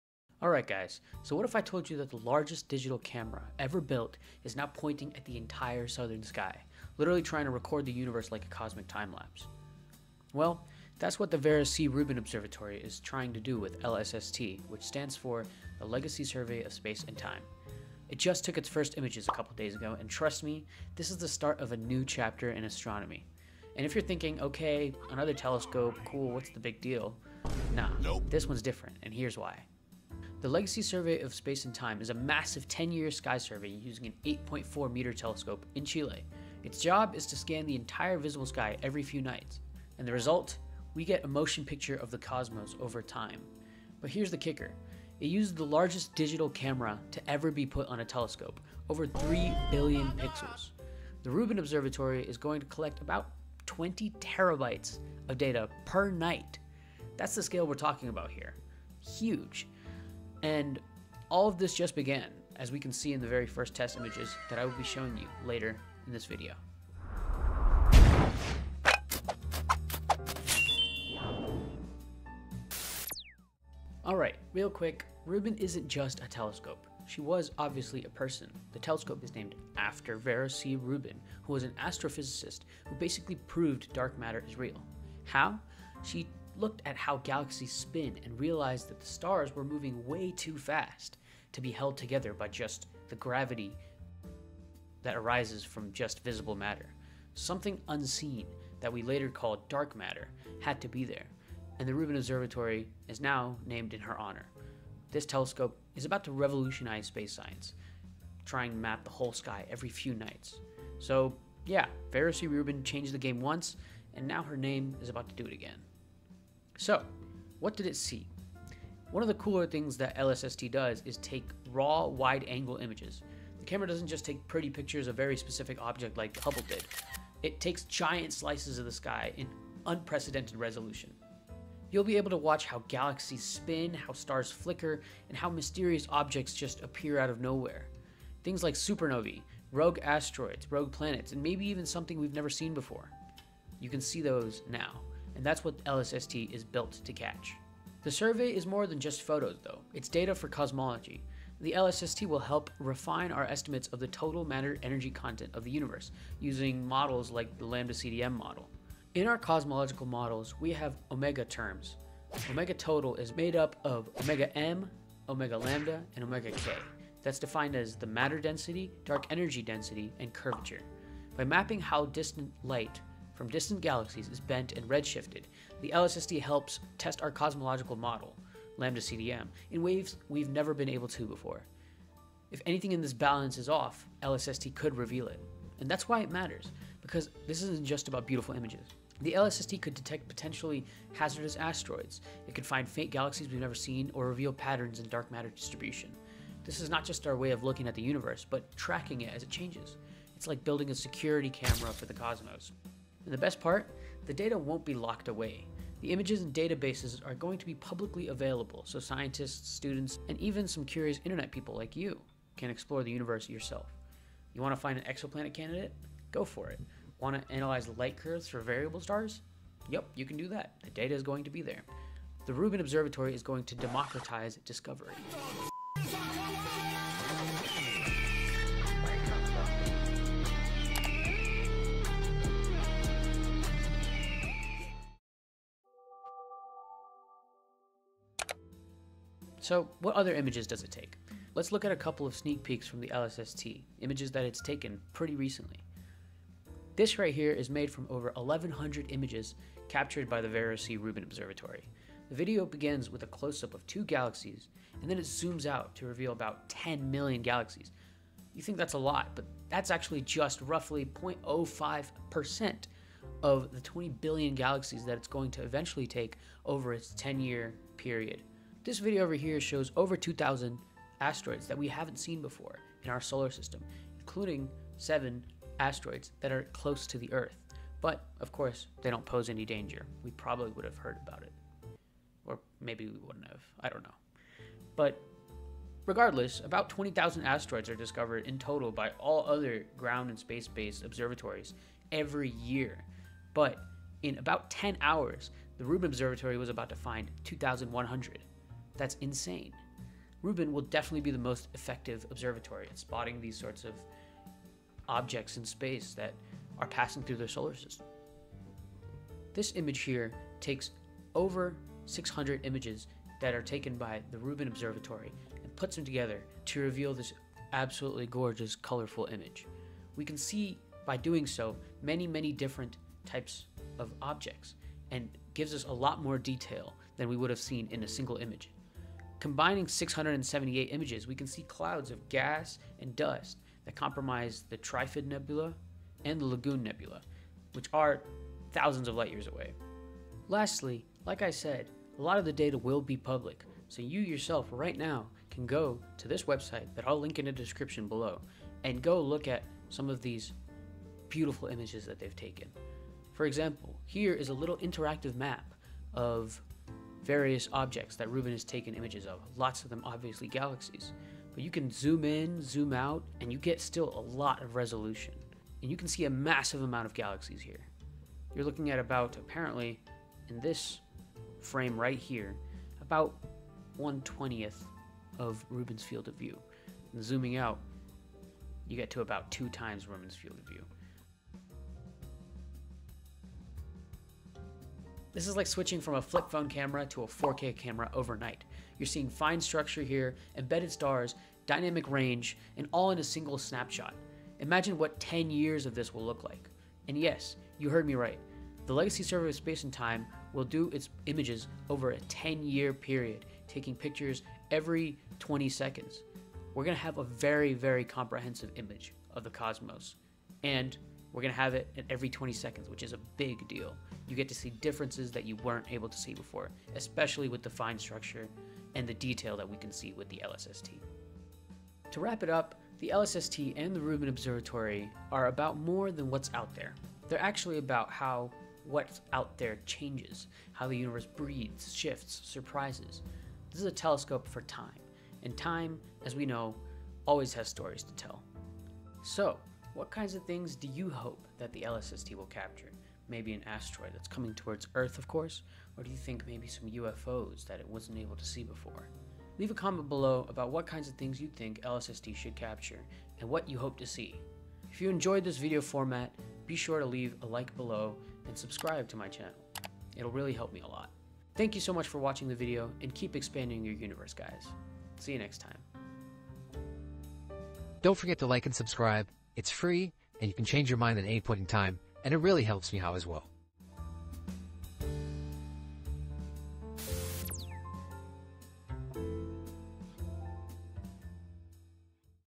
The LARGEST camera EVER just took its first pictures!
This groundbreaking observatory just captured its very first images—and they’re stunning. With a camera of unprecedented scale, it’s set to change how we map and explore the universe.

With these first pictures, the largest camera in the world is giving us a glimpse into the future of space science. Here's why the Rubin Observatory and its LSST will change how we see the entire universe.